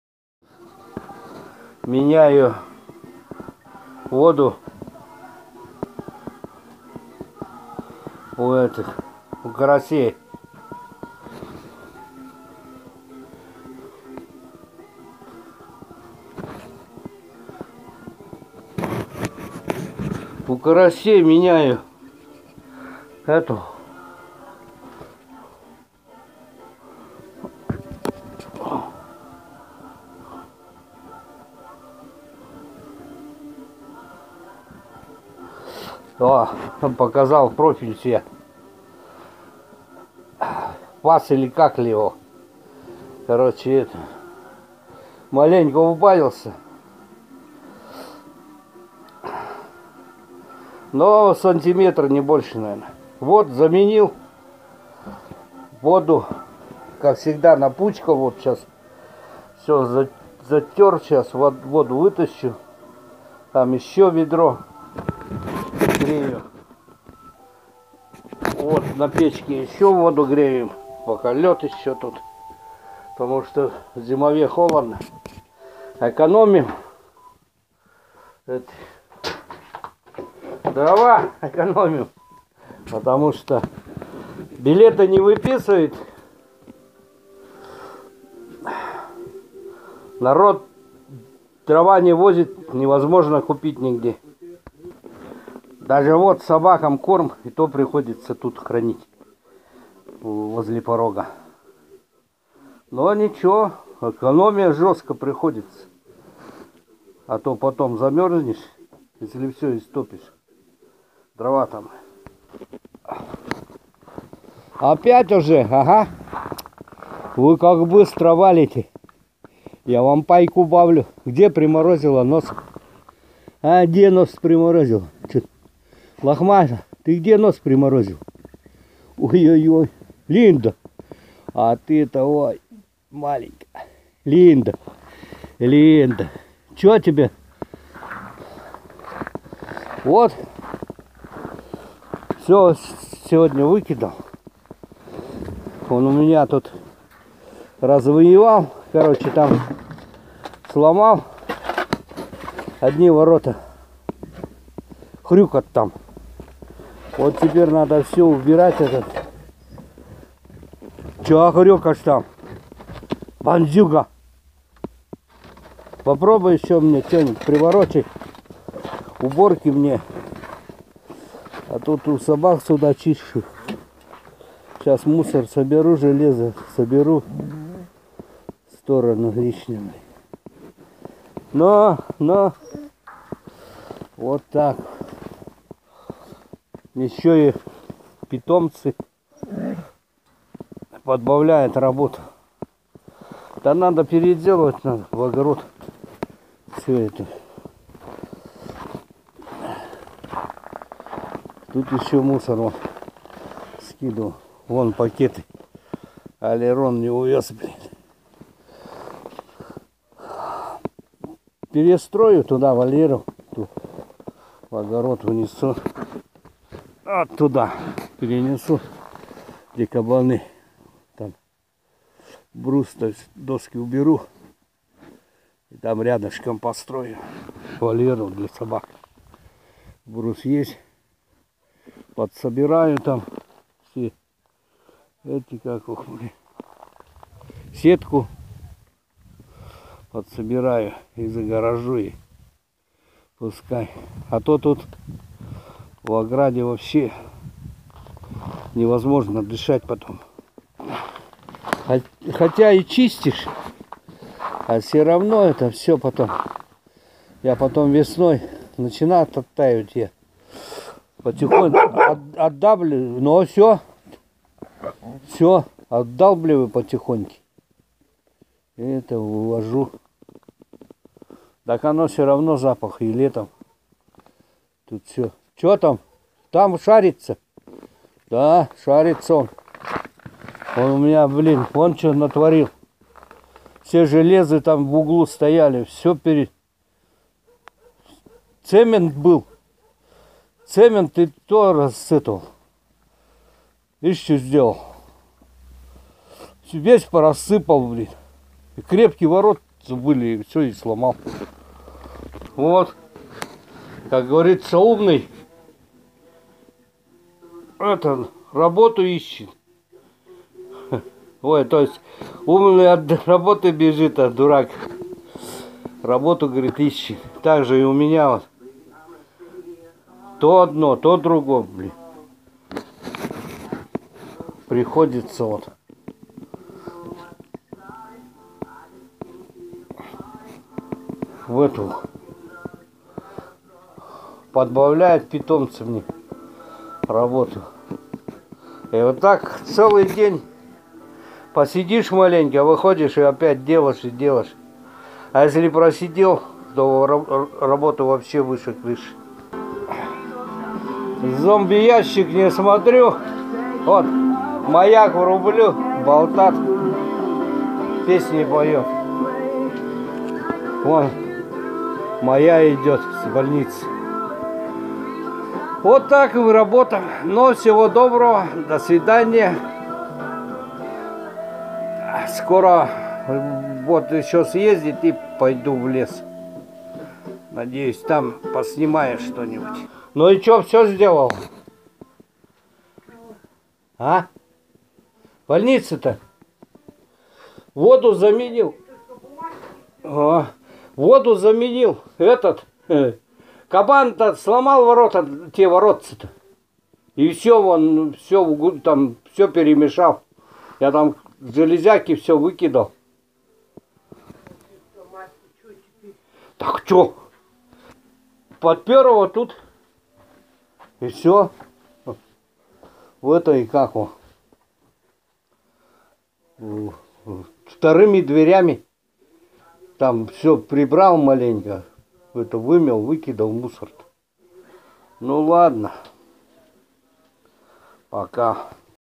От радости кабан, мне устроил бардак, адали медведь !!!
Я хочу вам  показывать Забайкальский говор  Адали  медведь . (Это словно медведь) просто иногда проскивают слова  ,наши чо ,за место что , понимаш . Да много других.Так что прошу не обижатся. !!